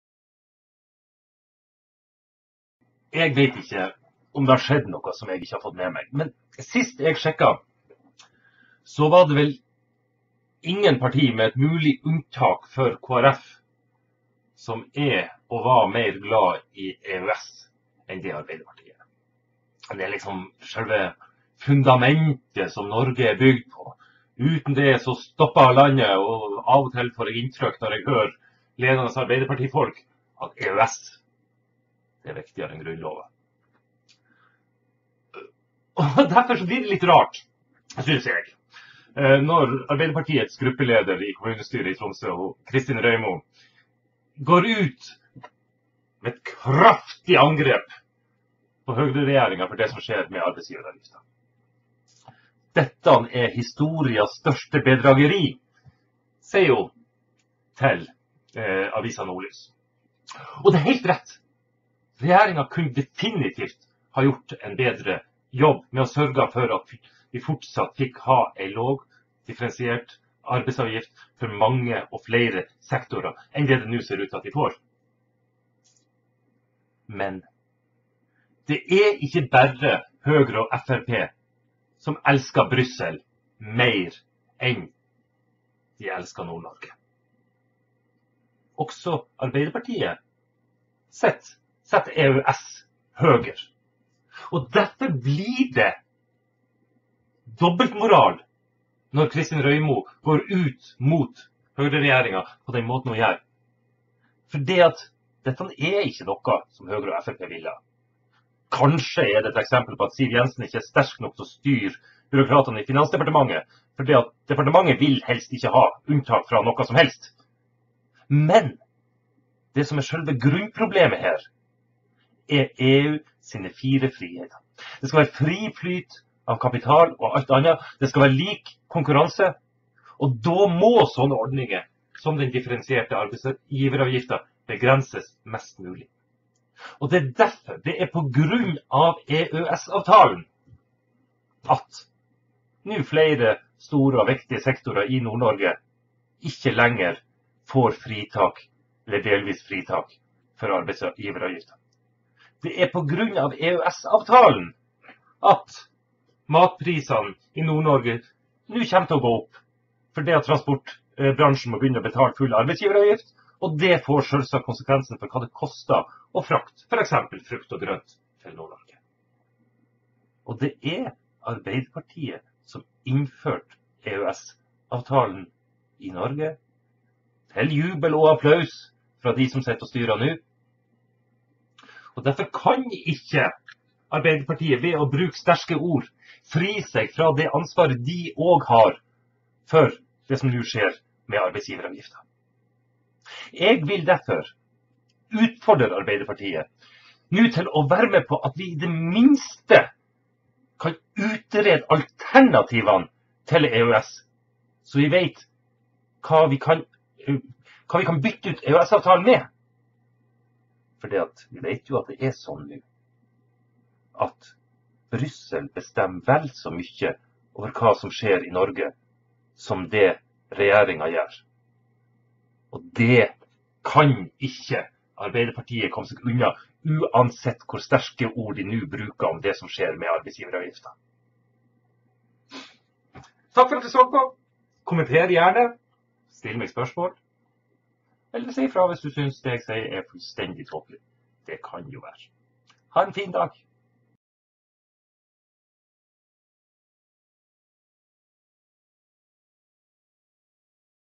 Arbeidsgjevaravgifta, eit bedrag frå fleire enn regjeringa
Aps Kristin Røymo går i Nordlys kraftig ut mot høgreregjeringa sitt knefall for EU. Ho kritiserer med full rett at regjeringa ikkje har gjort nok for å forhindre den store auken i arbeidsgjevaravgift for næringslivet i nord.

Røymo seier til Nordlys: «- Dette er historiens største politiske bedrageri.» Dessverre er Røymos utspel like mykje bedrageri som det høgreregjeringa driv med. Det er ikkje berre Høgre og Frp som elskar Brüssel meir enn Nord-Noreg, også Arbeidarpartiet har konsekvent prioritert Brüssel og EØS-avtalen høgare enn kva som er best for landsdelen vår og menneska i Noreg. Derfor er det ikkje berre litt dobbeltmoral som kjem til uttrykk i utspelet frå gruppeleiaren i Arbeidarpartiet.

Ja, høgreregjeringa kunne gjort meir. Men til grunn for desse avgiftsaukene ligg marknadstvangen i EUs fire «fridomar». Desse fridomane, som seier at kapitalen skal bevege seg fritt er det som gjer at vi får denne avgiftsauken.

Derfor, ikkje før Arbeidarpartiet støttar SV i at vi i det minste skal greie grundig ut kva som er alternativa til EØS-avtalen kan dei kritisere regjeringa i denne saka utan ein flau smak på tunga.

Her finn du i all hovudsak videobloggar og videoar med eit politisk tilsnitt, laga og publisert på bloggen "Eit stykkje av verda"